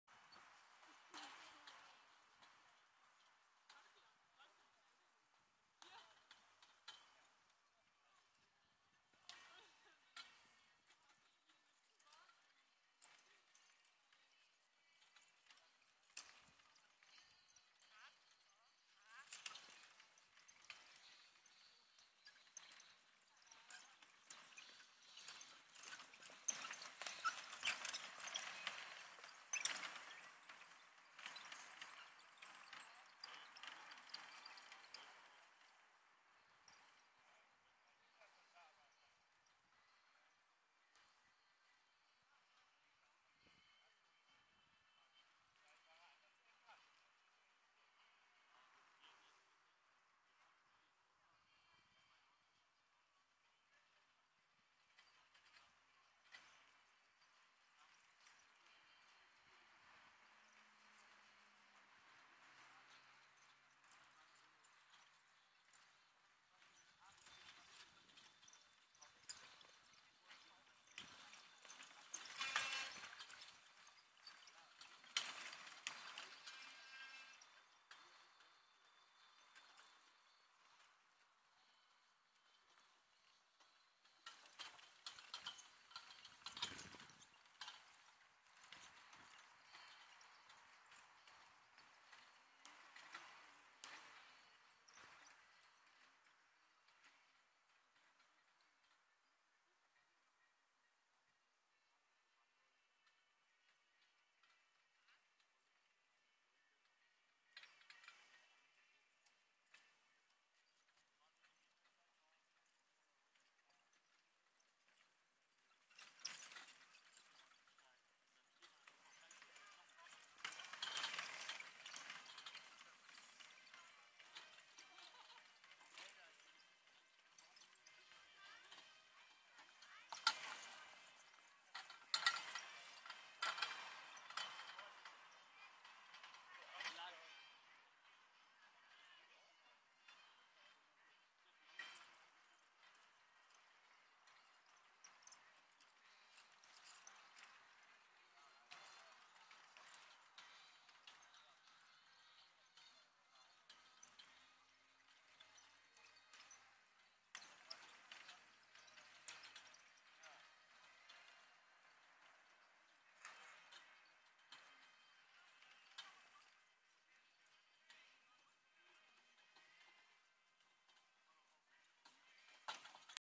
Fahrturnier Herford 8.8.2020
Ein kurzes Video einer Kegel-Fahr-Prüfung vom Fahrturnier in Herford
Samstag
Dir gefällt was ich mache?